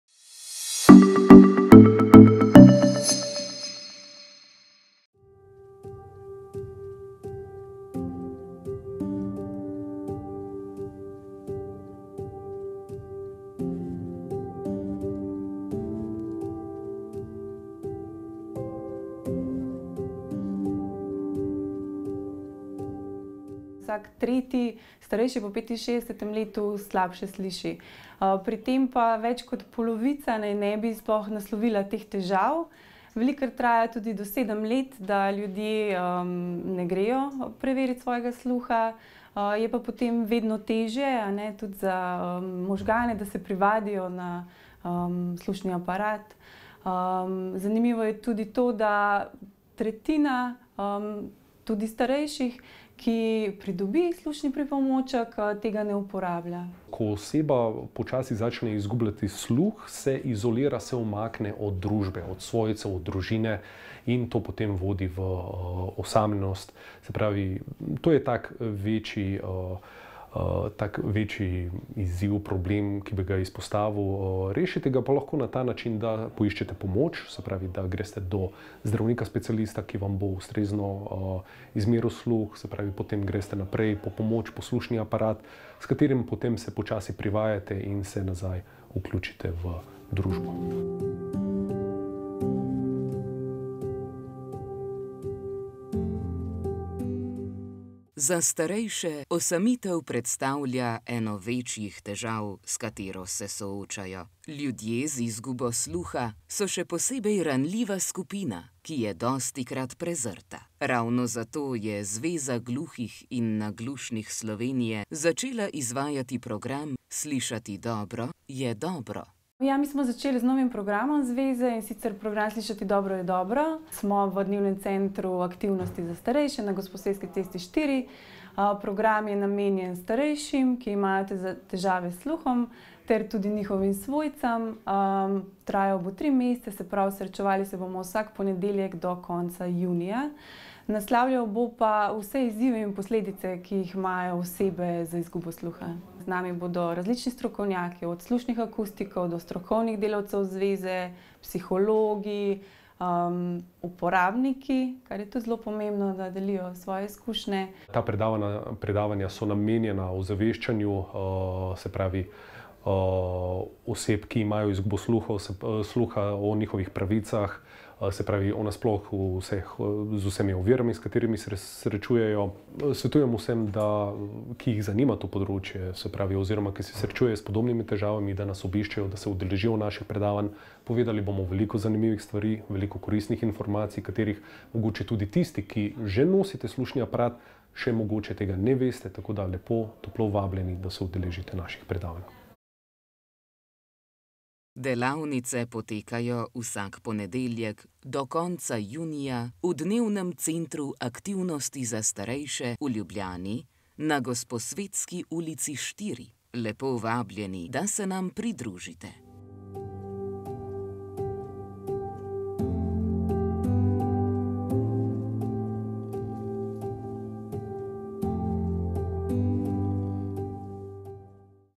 Slišati dobro je dobro
Vsaka tretja oseba po 65. letu starosti naj bi imela težave s sluhom. Več kot polovica ljudi teh težav ne rešuje. Ko nekdo počasi začne izgubljati sluh, se velikokrat odmakne od svojcev in prijateljev, se bolj zapira vase, kar lahko vodi v osamljenost. Tretjina starejših, ki dobi slušni aparat, slednjega sploh ne uporablja. Obiskali smo delavnico projekta »Slišati dobro je dobro«, ki je nastal pod okriljem Zveze gluhih in naglušnih Slovenije, namenjen pa je starejšim, ki se soočajo z izgubo sluha.